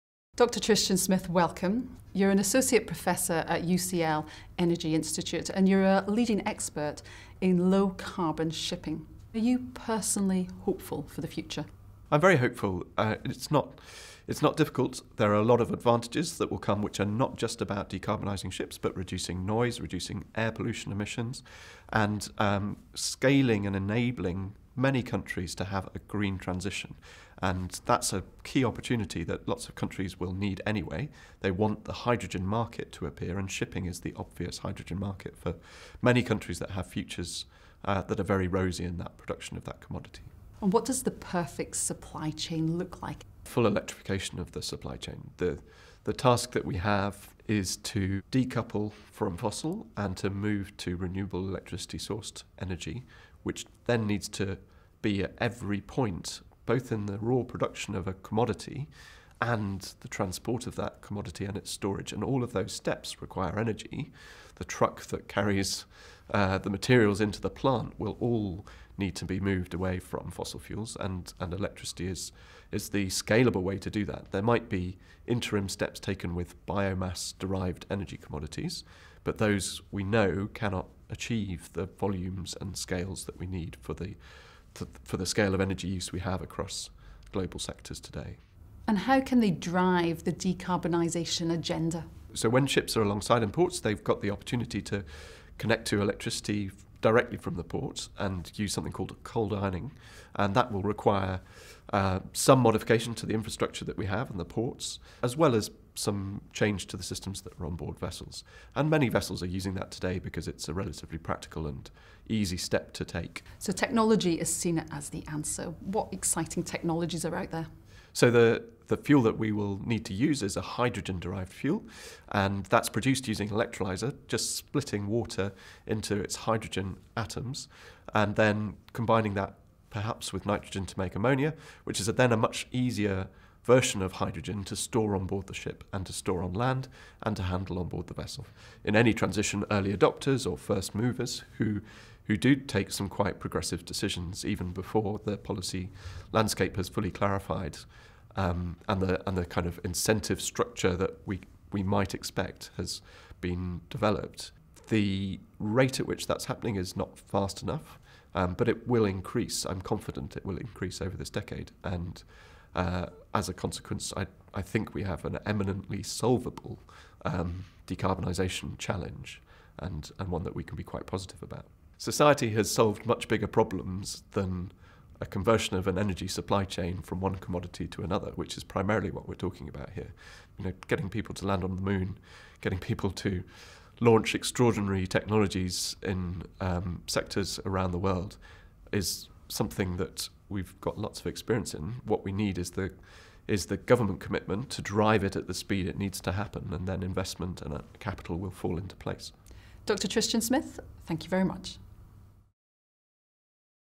Dr. Tristan Smith, Associate Professor, UCL Energy Institute | Gateways to Growth
Dr Tristan Smith, low carbon shipping expert at UCL Energy Institute explains why full electrification of the supply chain is the scalable way to move away from fossil fuels, and the implications this may have for ports.

'Gateways to Growth' is an online programme telling the story of Britain's ports and how they are adapting for the challenge of tomorrow.

Join in the conversation using